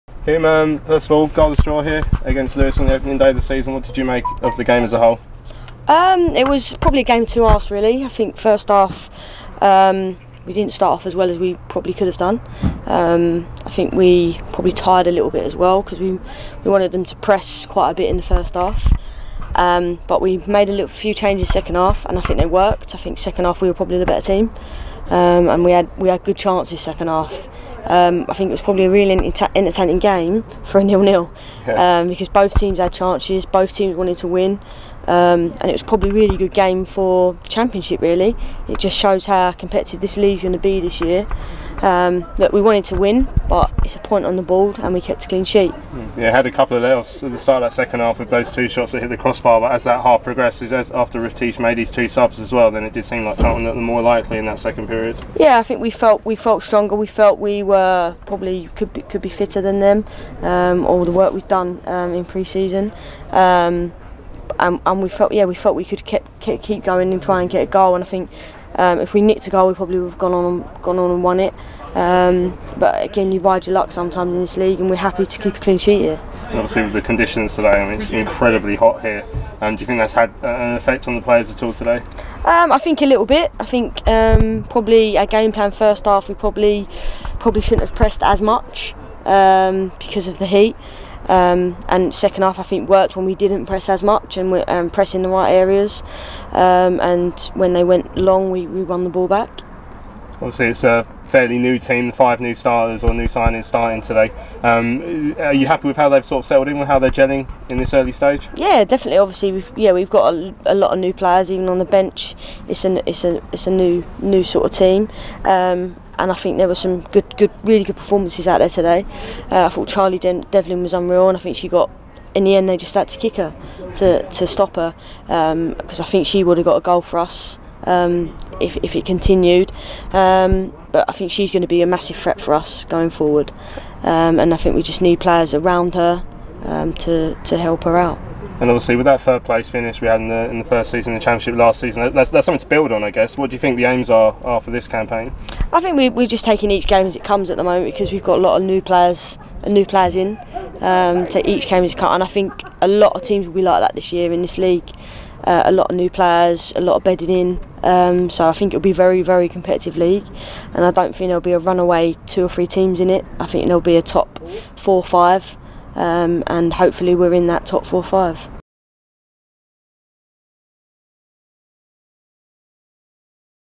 REACTION | Kim Dixson reacts to Charlton Athletic Women's 0-0 draw with Lewes
Assistant coach Kim Dixson reacts to Charlton's opening day draw with Lewes on Sunday 25 August.

The Addicks face London City Lionesses in their next home game on Sunday 15th September.